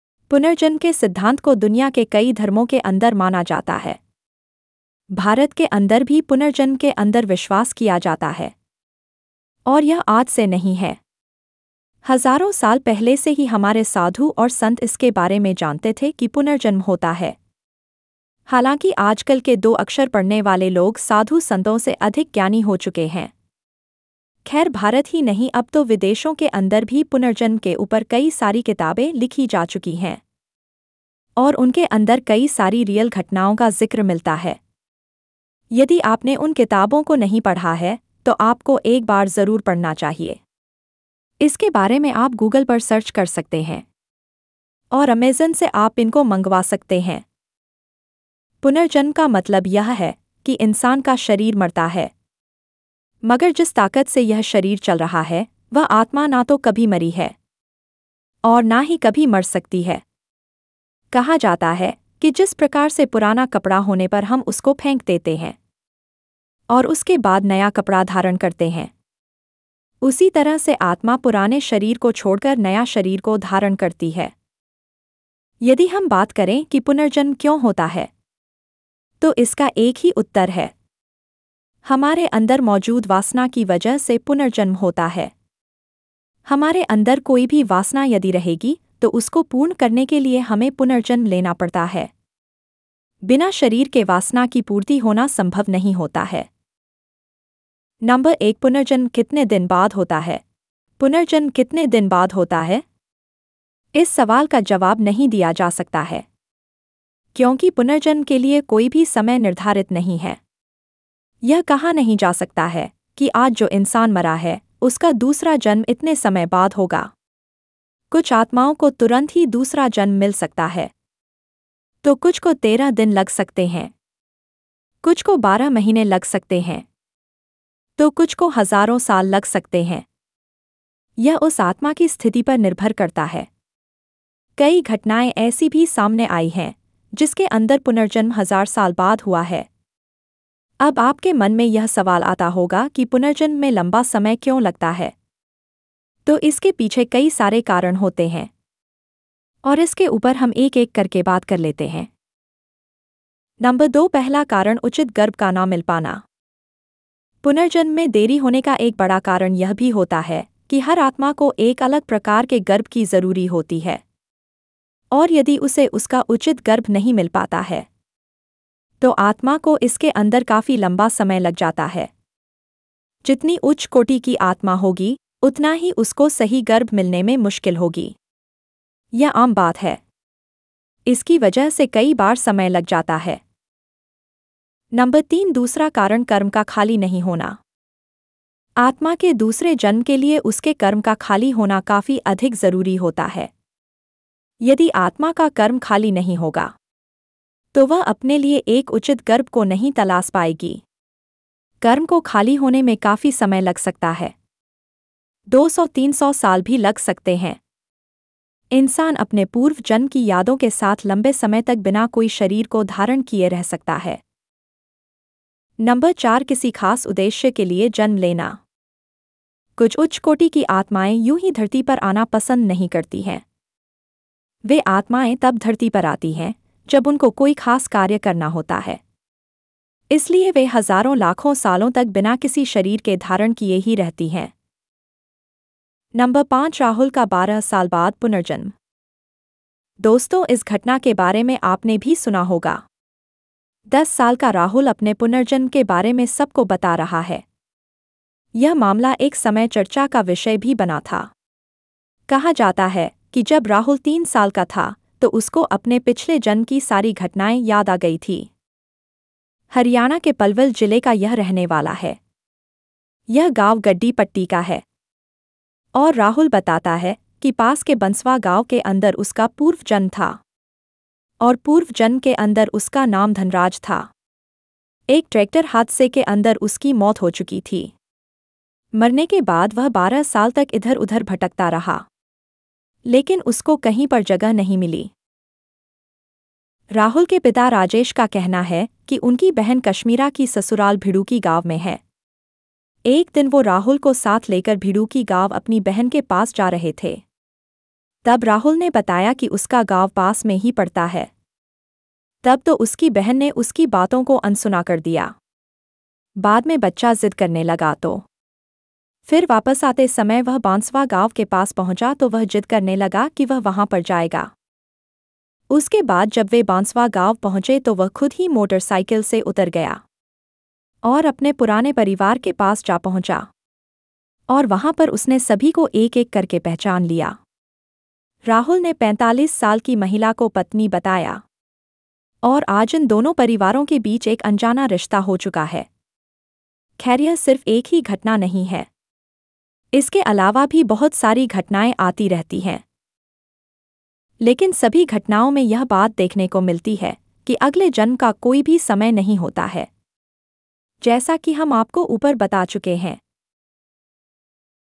पुनर्जन्म कितने दिन बाद होता है /punarjanam kitne din baad hota hai
पुनर्जन्म कितने दिन बाद होता है punarjanam kitne din baad hota hai पुनर्जन्म के सिद्धांत को दुनिया के कई धर्मों के अंदर माना जाता है। भारत के अंदर भी पुनर्जन्म  के अंदर विश्वास किया जाता है। और यह आज से नहीं है। हजारों साल पहले से ही हमारे साधु और संत इसके बारे मे जानते थे कि पुनर्जन्म  होता है